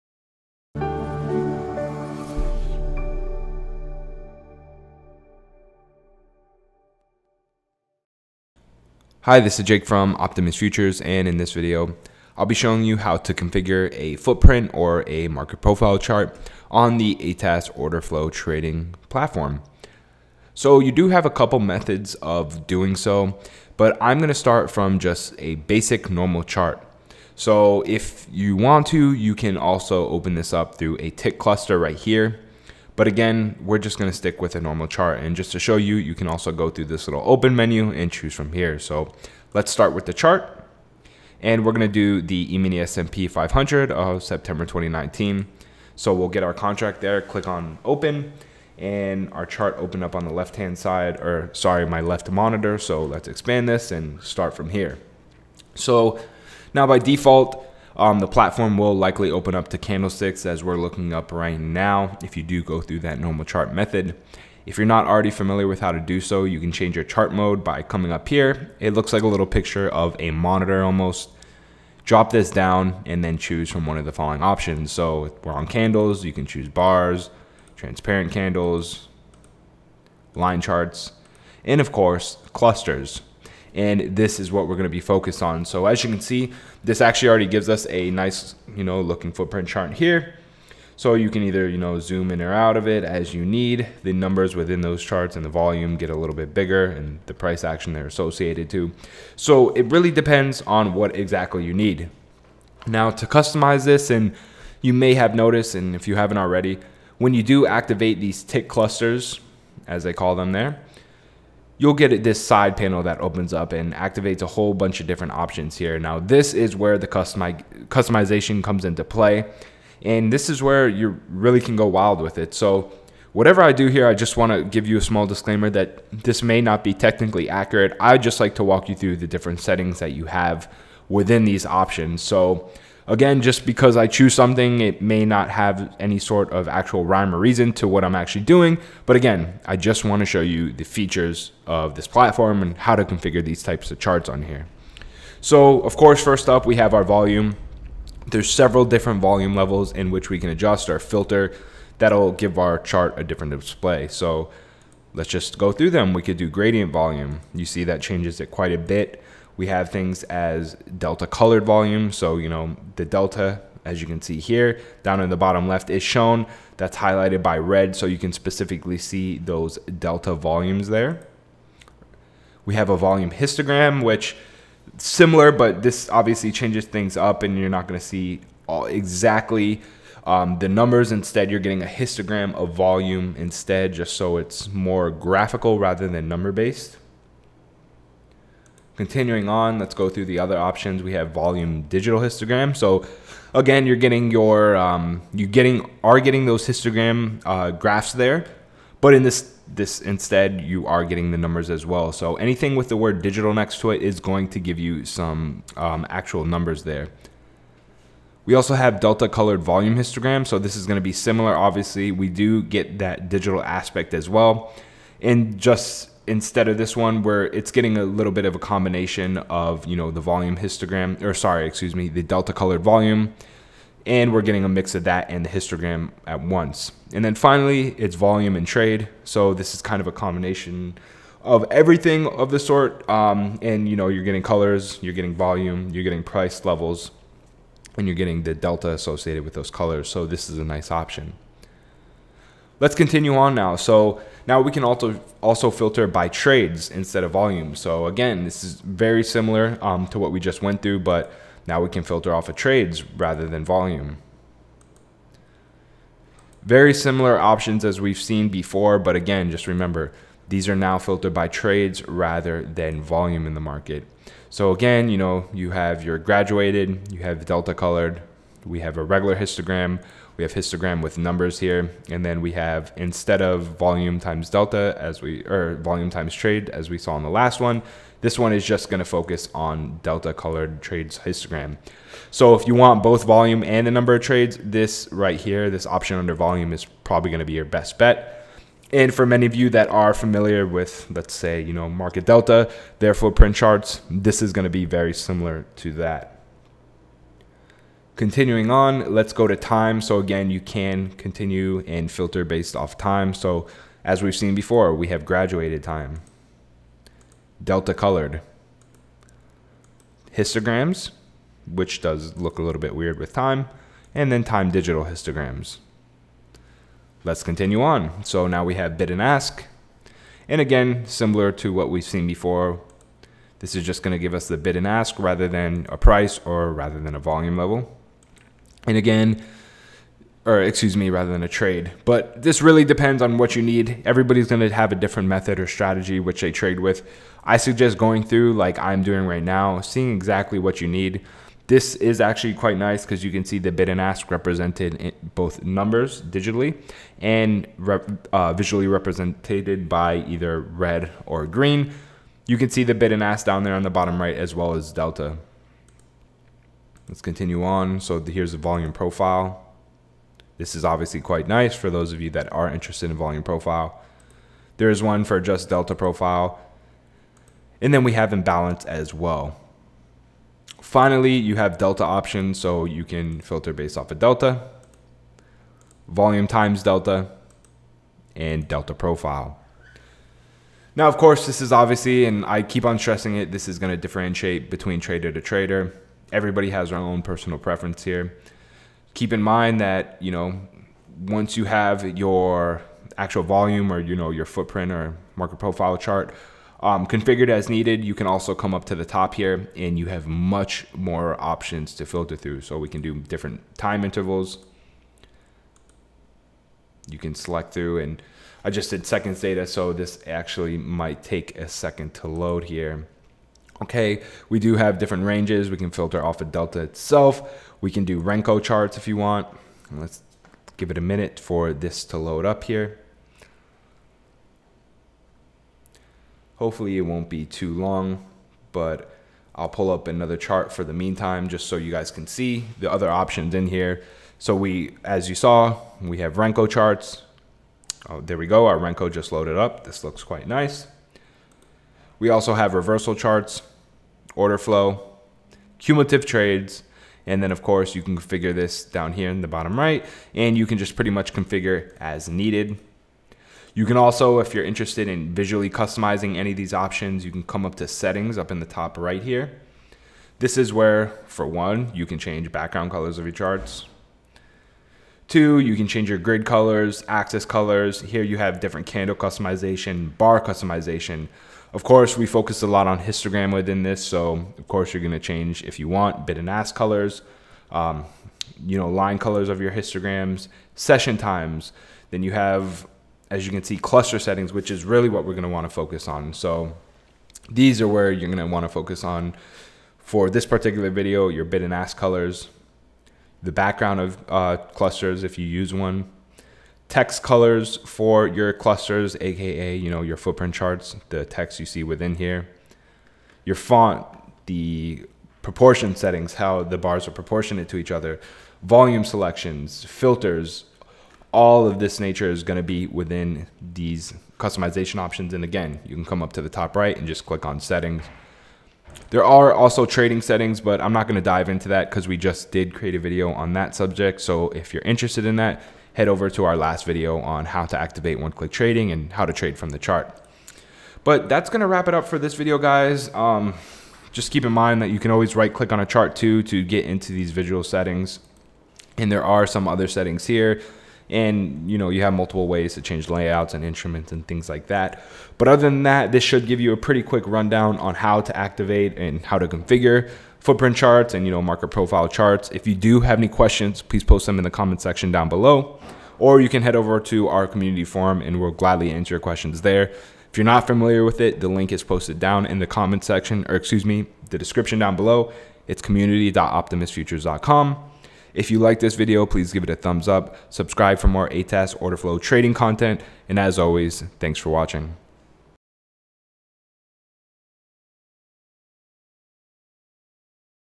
ATAS | Order Flow Trading - How to Configure Footprint and Market Profile Charts
The following video will demonstrate how to configure and customize footprint and market profile charts on the ATAS | Order Flow Trading Platform.